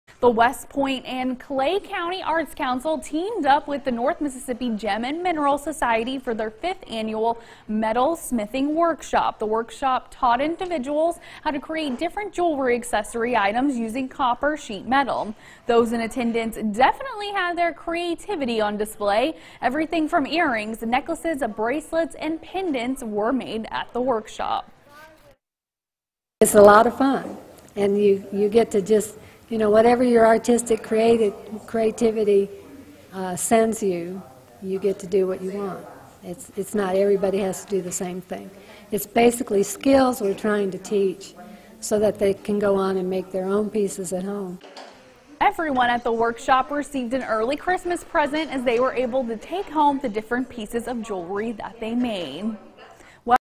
Metalsmithing
Clay County Arts Council and the North MS Gem and Mineral Society holds annual metal - smithing workshop